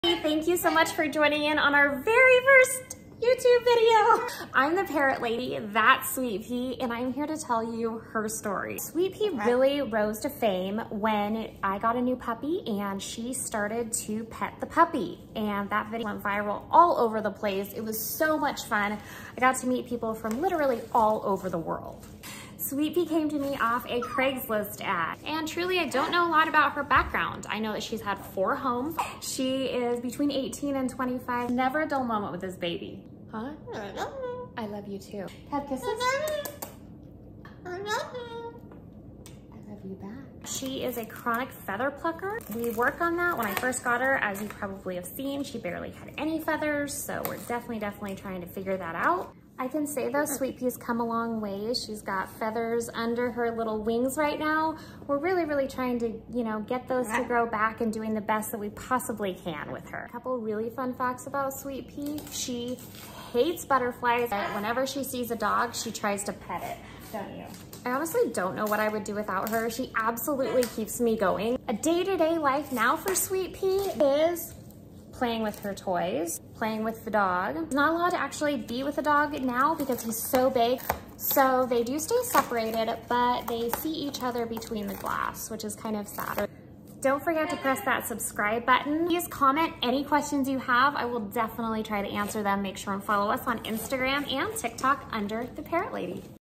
My first YouTube Video! Meet Sweet Pea, my Umbrella Cockatoo!
Sweet Pea is the sweetest of parrots. Check out the viral video of her meeting her new puppy brother for the first time in my Shorts!